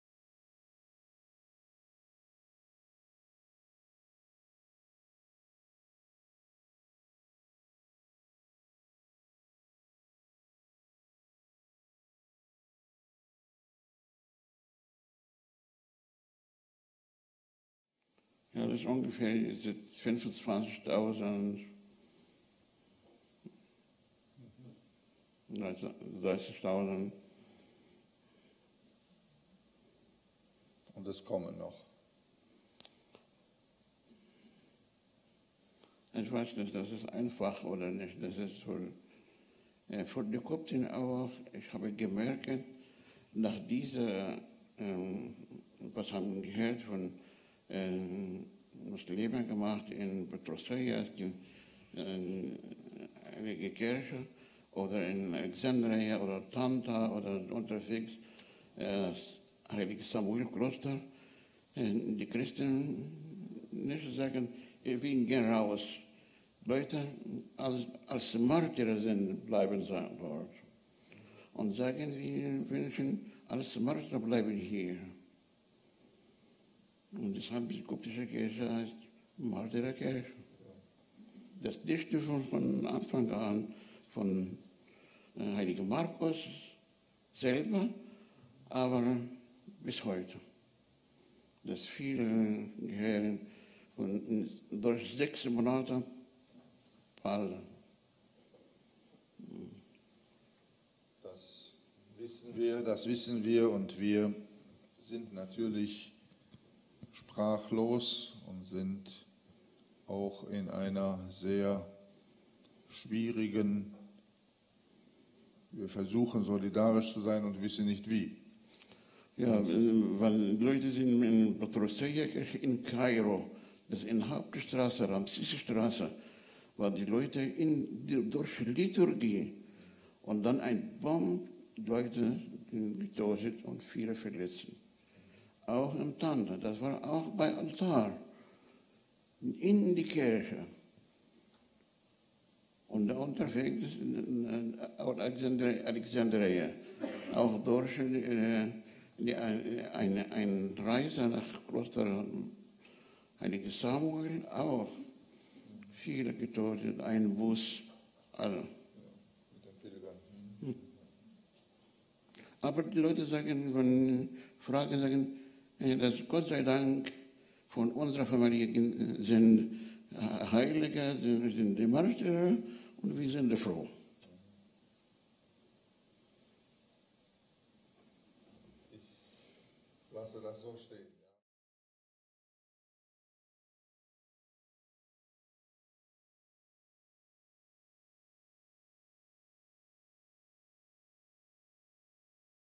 10.4 Terror gegen koptische Christen in Ägypten
Koptische Christen sehen sich in Ägypten auch Terror ausgesetzt.